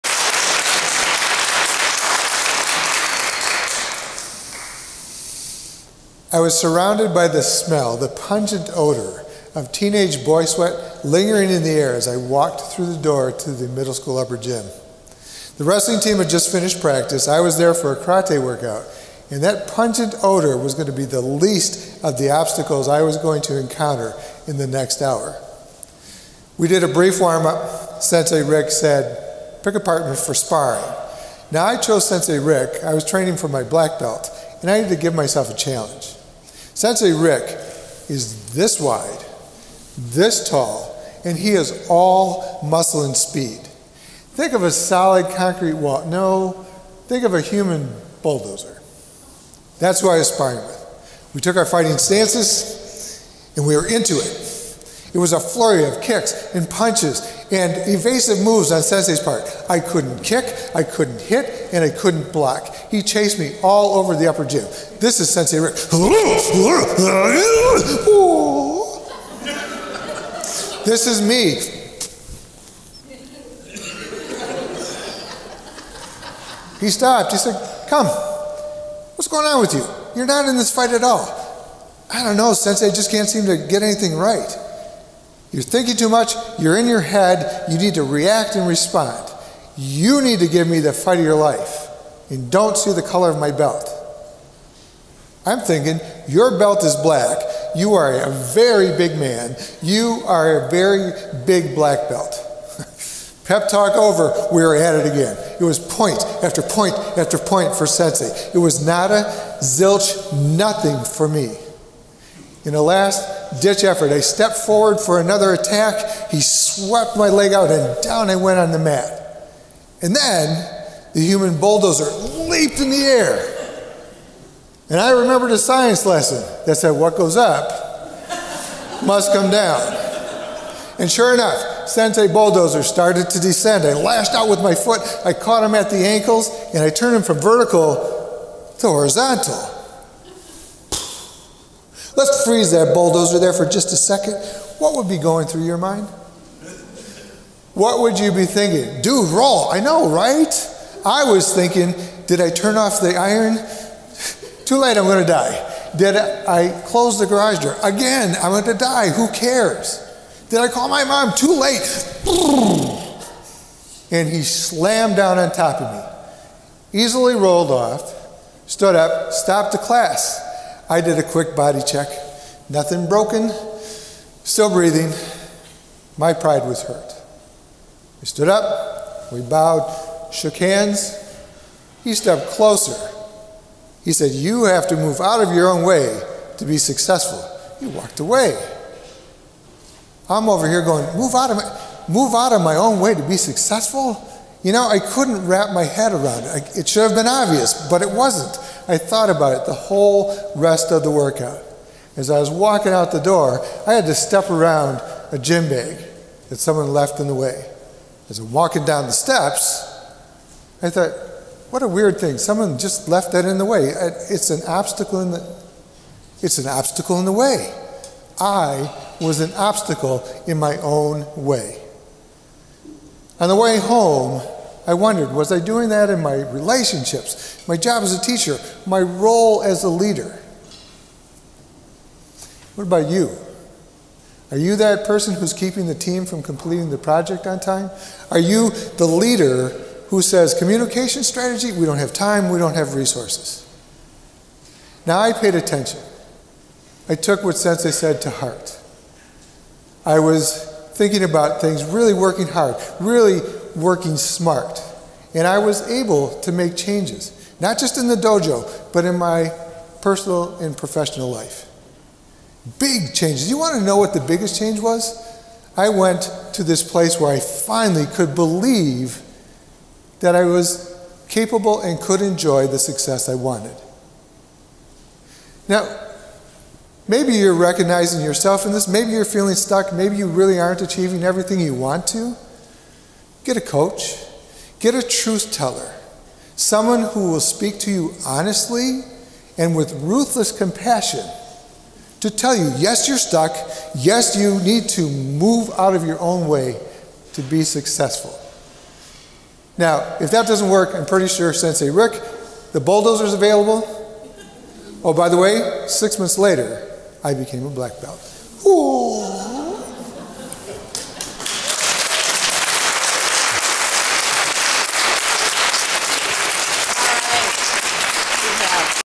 Sparring with Sensei-Move out of Your Own Way
Too often we get in our own way to create the kinds of success we want. The video is from the National Speakers Association Wisconsin Storytelling Competition held in Madison, WI on February 13th, 2024. Watch what happens in the telling of a sparring match between myself and my karate sensei.